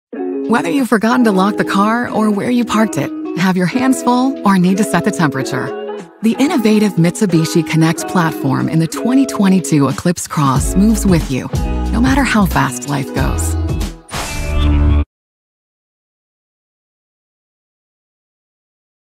Bell Road Mitsubishi 2022 Mitsubishi Eclipse Cross
Meet the radically redesigned 2022 Eclipse Cross and say goodbye to compromise. Featuring improved handling, bold design that rivals the most stylish SUVs, and tech that takes connectivity to the next level. Go where you want, when you want, exactly how you want with the new Eclipse Cross.